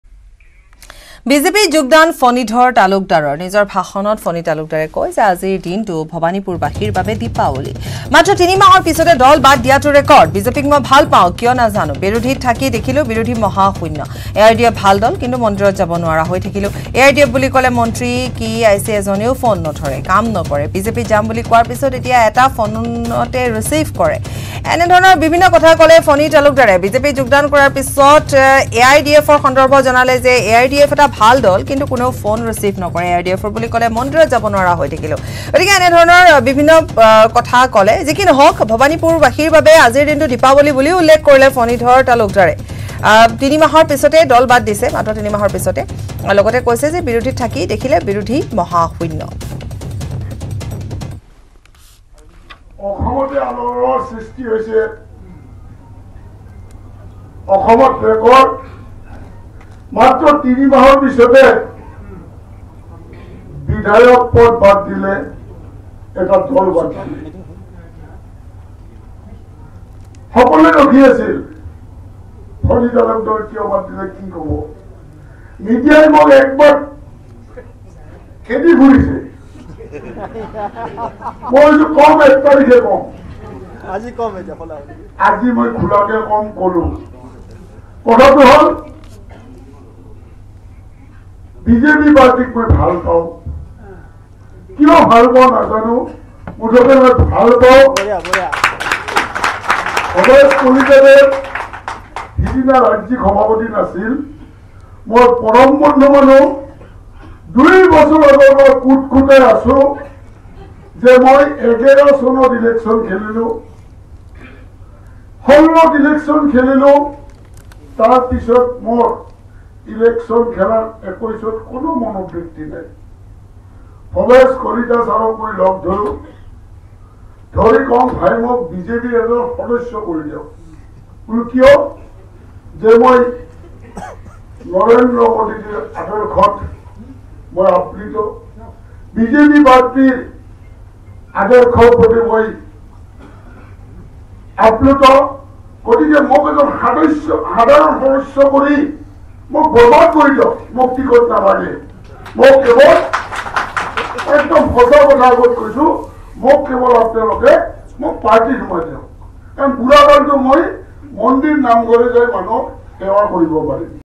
Phani Talukdar | আজিৰ দিনটো ভৱানীপুৰবাসীৰ বাবে দিপাৱলী
1 September, 2021: বিজেপিত যোগদান ফণীধৰ তালুকদাৰৰ। মাত্ৰ ৩ মাহৰ পাছতে দল বাদ দিয়াটো ৰেকৰ্ড